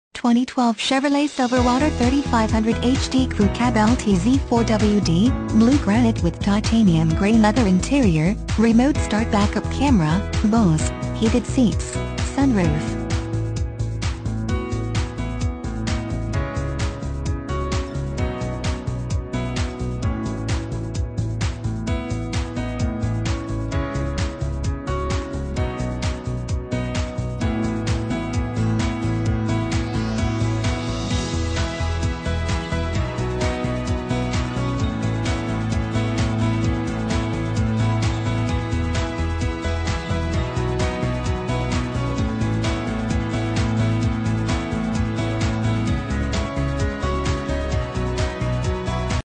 2012 CHEVROLET SILVERADO 3500HD Woods Cross, UT T25329
2012 CHEVROLET SILVERADO 3500HD Woods Cross, UT
Stock

Murdock Chevrolet
2375 S 625 W Woods Cross

2012 Chevrolet Silverado 3500HD Crew Cab LTZ 4WD  - Blue Granite with titanium gray leather interior - remote start  backup camera - BOSE - heated seats - SUNROOF